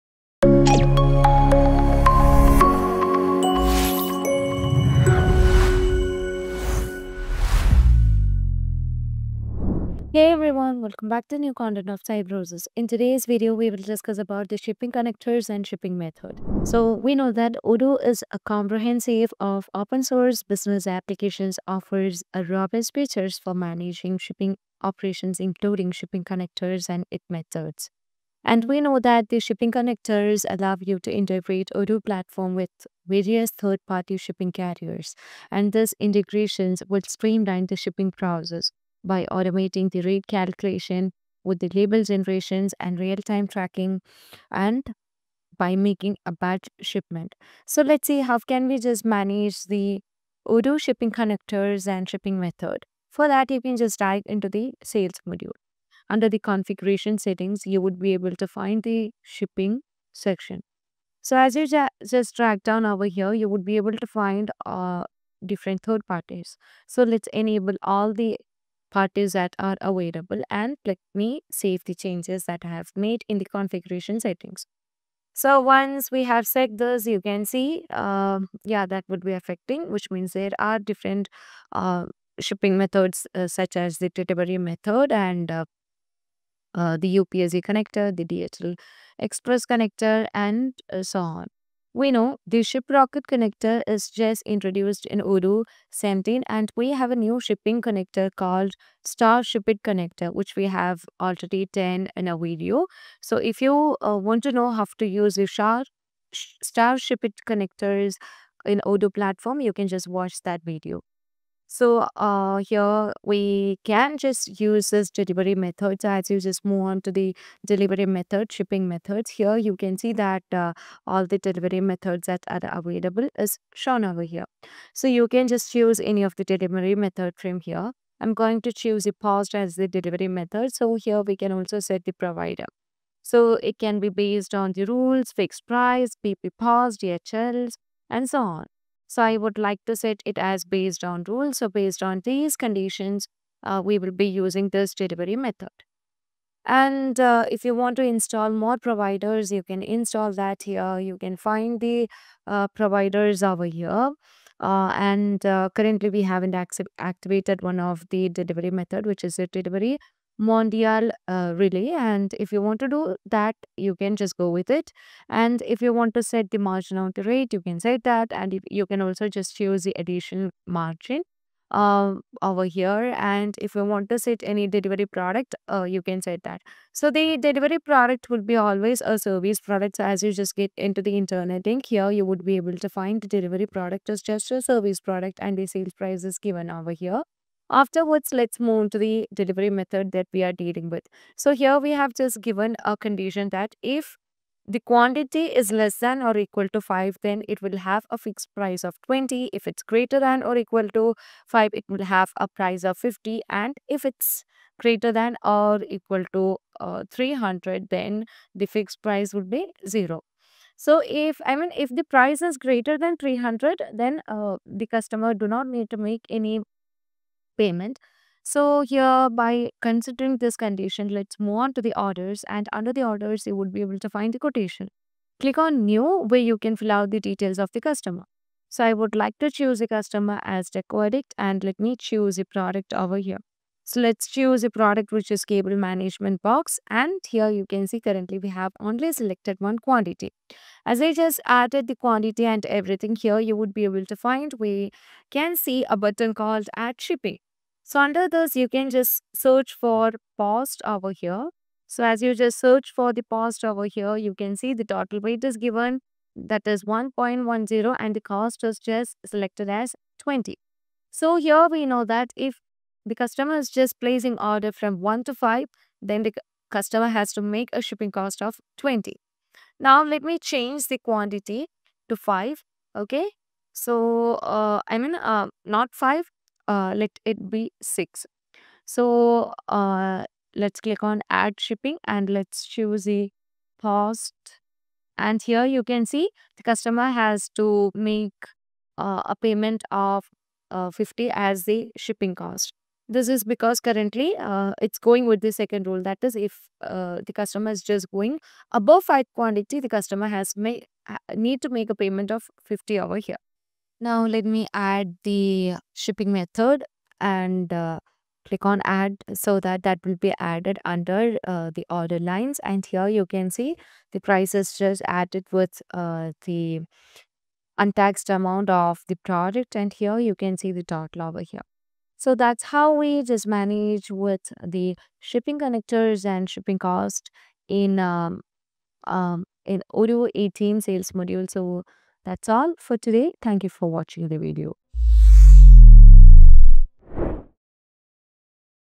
How to Manage Shipping Connectors & Shipping Method in Odoo 18 Sales | Odoo 18 New Features
Shipping Connectors and Methods in Odoo 18 Sales

Odoo 18 offers a robust system for managing shipping, both through internal methods and external shipping carriers. This guide will delve into both aspects:

1. Internal Shipping Methods

Configuration:

Navigate to Inventory -- Configuration -- Settings.
Under the Delivery Methods section, create new methods as needed.
Define the following for each method:
Name: A descriptive name for the method (e.g., "In-house Delivery").
Carrier: Select "Manual" for internal methods.
Product: Choose the product or product category associated with this method.
Fixed Price: Set a fixed price for the shipping cost.
Weight-Based Price: Define weight ranges and corresponding prices.
Volume-Based Price: Define volume ranges and corresponding prices.
Delivery Lead Time: Specify the estimated delivery time.

Usage:

When creating a sales order, select the desired internal shipping method.
The system will automatically calculate the shipping cost based on the defined rules.

2. Third-Party Shipping Connectors

Odoo integrates with various third-party shipping carriers to streamline the shipping process.

Configuration:

Install the necessary shipping connector module (e.g., DHL, FedEx, UPS).
Configure the connector with your carrier account credentials.
Define shipping methods within Odoo, linking them to the respective carrier and service.
Set up product weights and dimensions to accurately calculate shipping costs.

Usage:

When creating a sales order, select the desired third-party shipping method.
The system will fetch real-time shipping rates from the carrier's API.
You can choose the most suitable option and proceed with the order.

Additional Considerations:

Shipping Labels: Odoo can generate shipping labels directly from the sales order or delivery order.
Tracking Information: You can track shipments within Odoo by integrating with carrier tracking APIs.
Shipping Cost Invoicing: Odoo can automatically invoice shipping costs to customers.
Multiple Packages: You can split orders into multiple packages and assign different shipping methods to each package.
Dropshipping: Odoo supports dropshipping workflows, where the supplier directly ships products to the customer.
By effectively utilizing both internal and external shipping methods, you can optimize your shipping processes, reduce costs, and improve customer satisfaction.